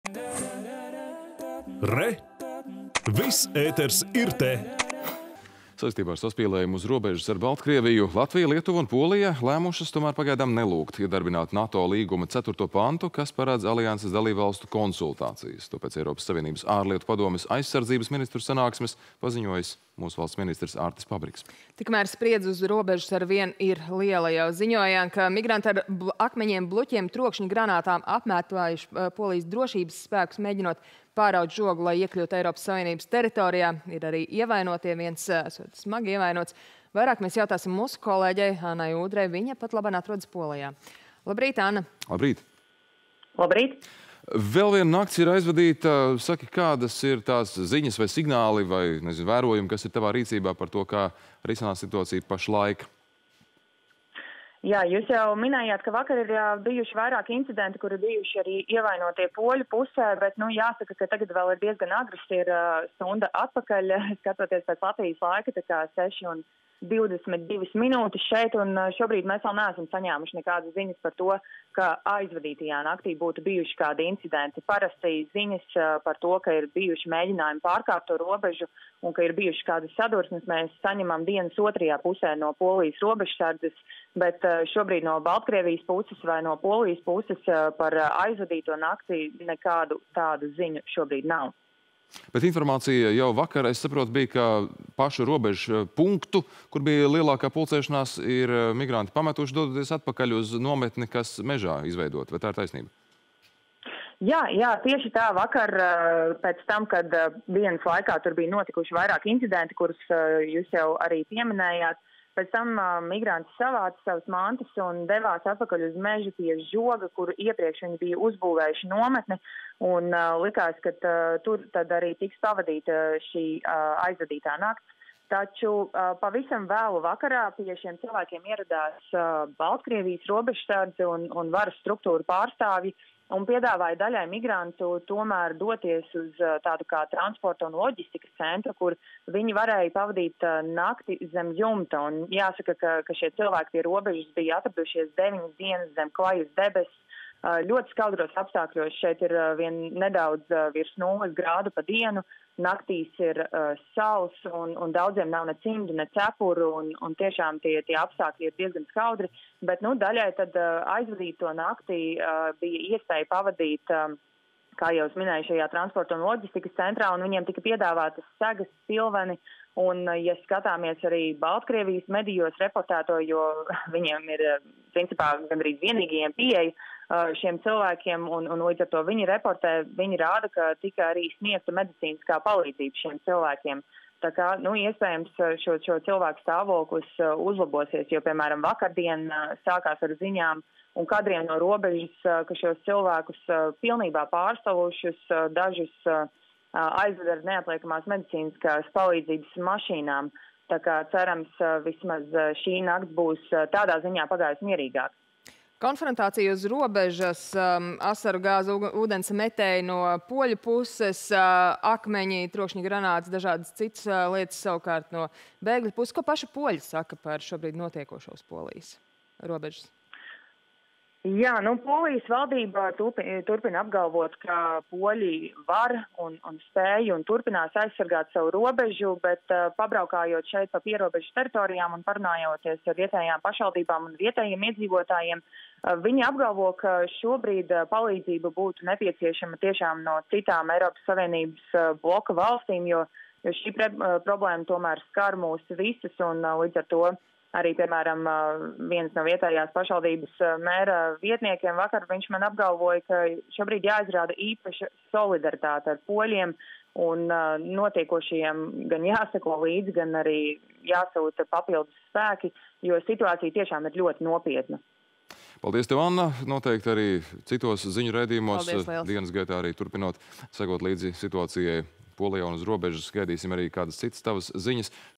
Jaunākā situācija uz Baltkrievijas - Polijas robežas
Saistībā ar saspīlējumu uz robežas ar Baltkrieviju / Latvija, Lietuva un Polija lēmušas pagaidām nelūgt iedarbināt NATO līguma 4.pantu, kas paredz alianses dalībvalstu konsultācijas. To pēc Eiropas Savienības Ārlietu padomes aizsardzības ministru sanāksmes paziņojis ministrs Artis Pabriks.
    Tikmēr spriedze uz robežas arvien ir liela – jau ziņojām, ka migranti
ar akmeņiem, baļķiem un trokšņa granātām apmētājuši Polijas drošības spēkus, mēģinot pārraut žogu, lai iekļūtu ES teritorijā. Par jaunāko – vaicāsim kolēģei Annai Ūdrei Polijā.